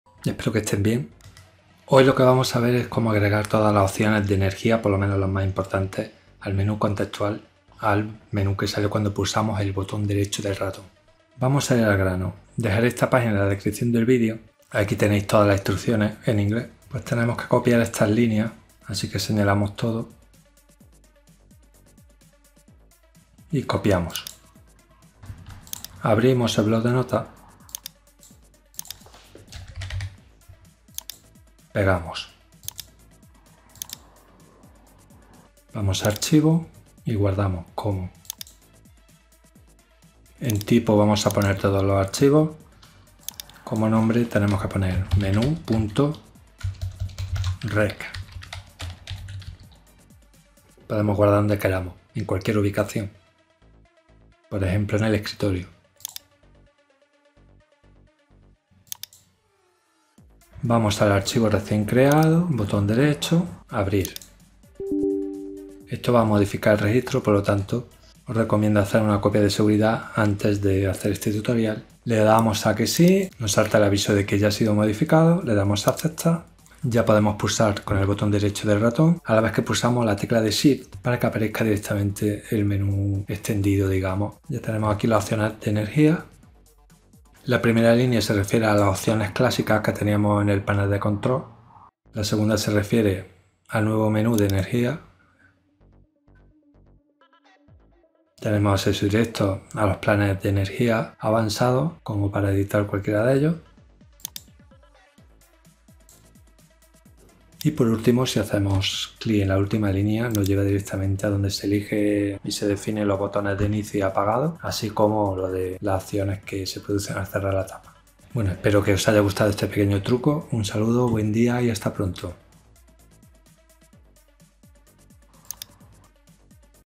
Agregar opciones de energía al menú contextual del clic derecho
veremos cómo añadir un útil menú contextual de Opciones de energía al Escritorio. Permitirá acceder rápidamente a varias opciones de energía y ajustar la configuración actual del plan de energía. Es más rápido que cambiarlos en Configuración o usar el Panel de control clásico.

Para agregar las opciones de energía al menú contextual del clic derecho, puedes seguir estos pasos: Ejecuta el Bloc de notas, copia y pega el contenido del ajuste del Registro proporcionado en la web en un nuevo archivo de texto y ejecuta el archivo para modificar el Registro. Esto agregará un menú contextual de Opciones de energía al escritorio, permitiéndote acceder rápidamente a varias opciones de energía y ajustar la configuración de tu plan de energía actual.

Nota:
Asegúrate de crear una copia de seguridad del Registro antes de realizar cualquier cambio.
Si tienes problemas, puedes restaurar el Registro a su estado anterior utilizando la copia de seguridad.

Add Power Options Context Menu in Windows

Música de fondo by @JJ_Millon